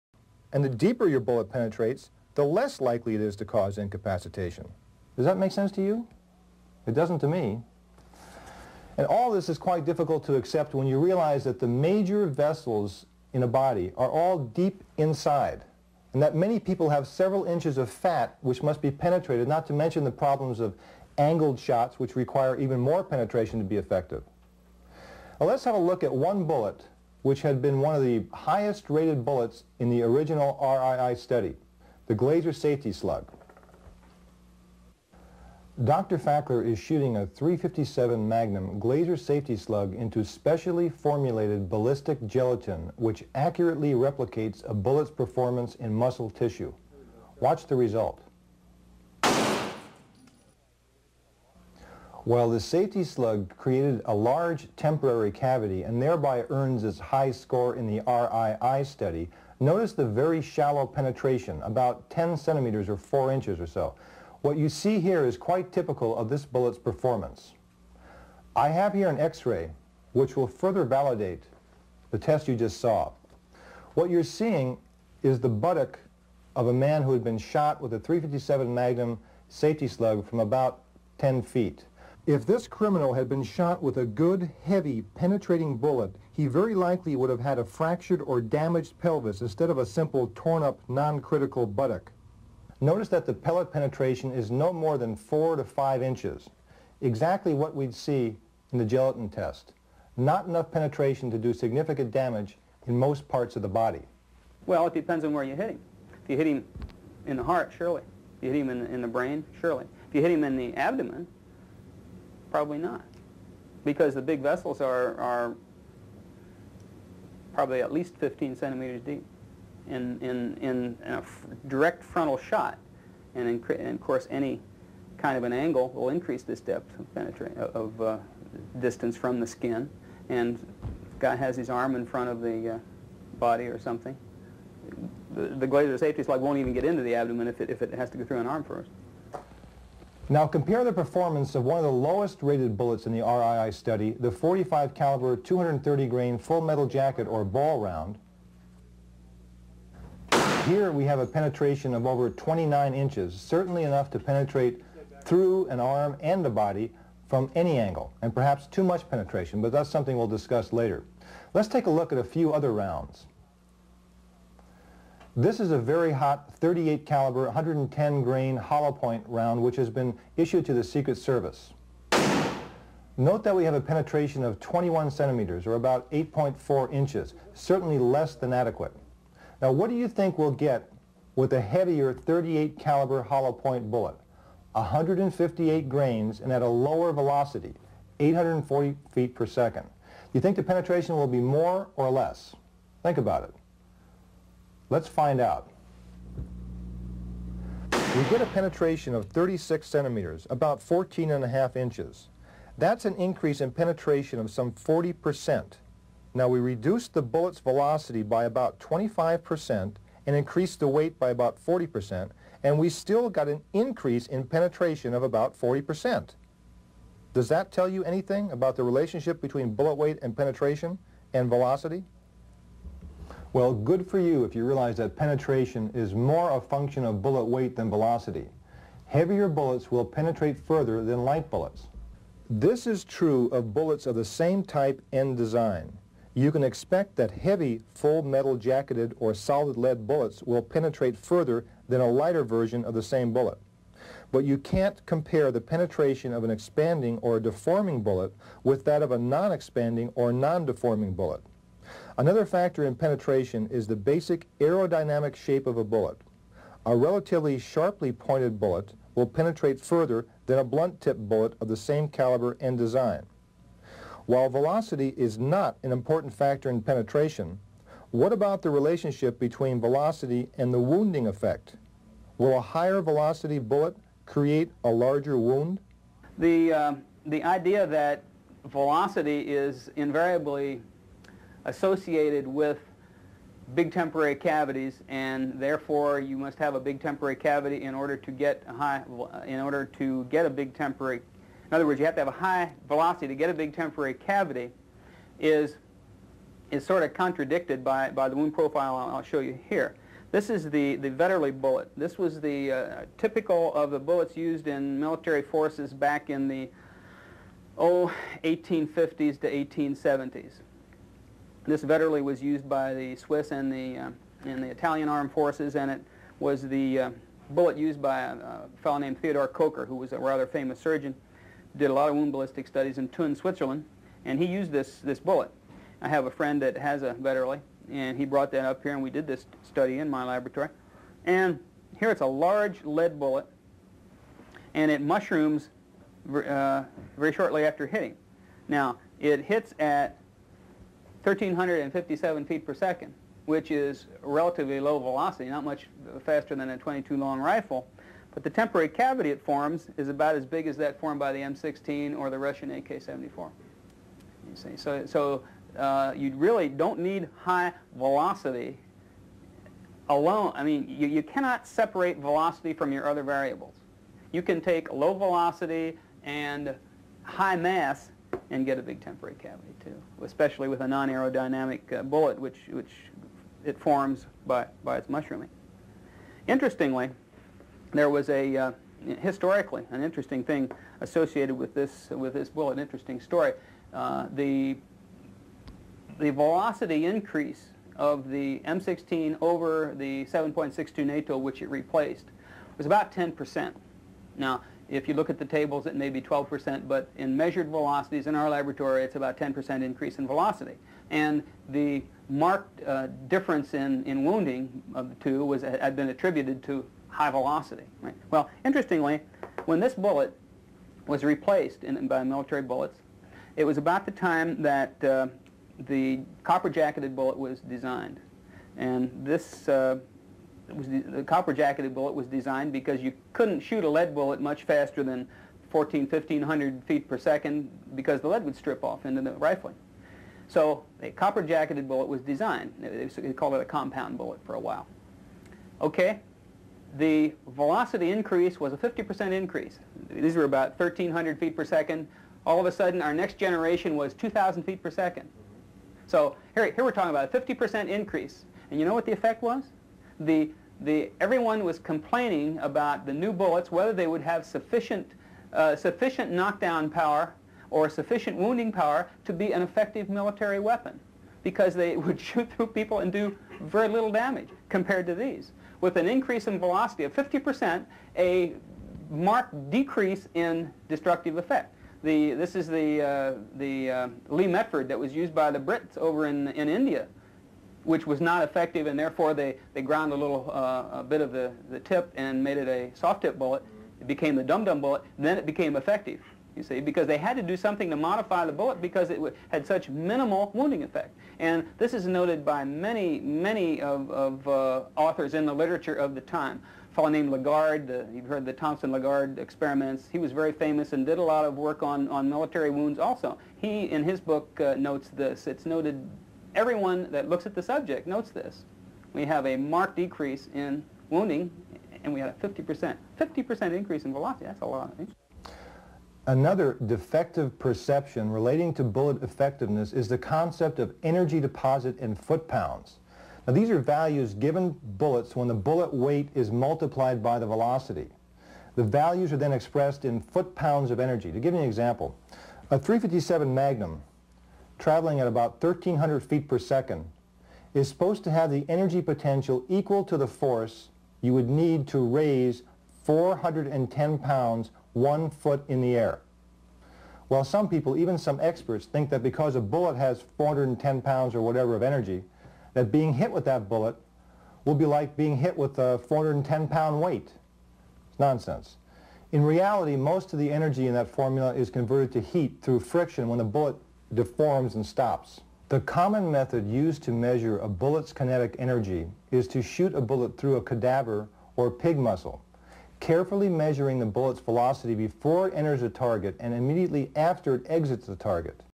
BALISTICA TERMINAL - parte 3
Na terceira parte ele fala da ineficiência de munições pré-fragmentadas do tipo glaser, magsafe, etc. O motivo principal é a falta de penetração adequada.
Fala também da relação da velocidade e peso na penetração. E os efeitos da penetração no ferimento.